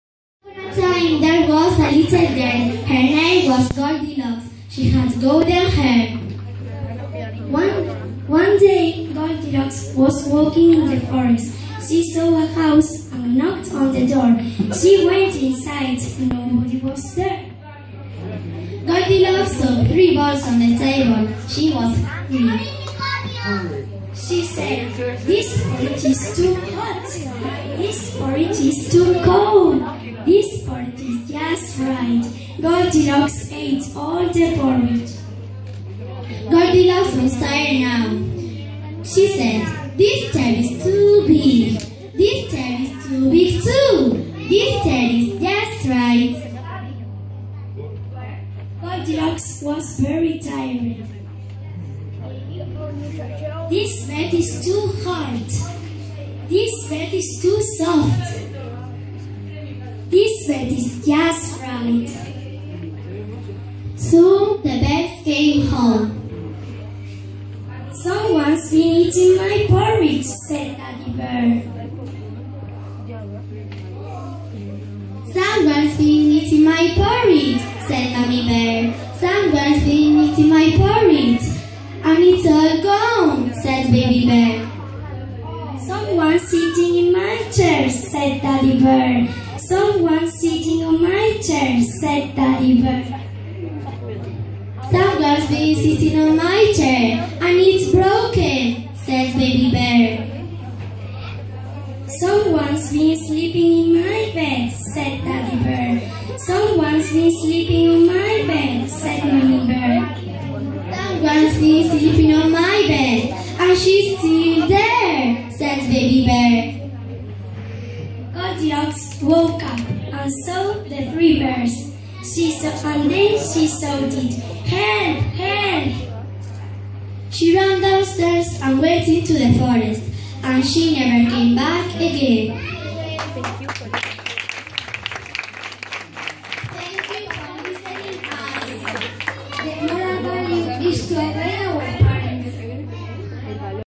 Cuentacuentos Ricitos de oro y los tres osos
Cuentacuentos Ricitos de oro y los tres osos realizado con motivo del proyecto de inglés de los alumnos de 5º de Primaria del Colegio de La Presentación de Guadix (Granada)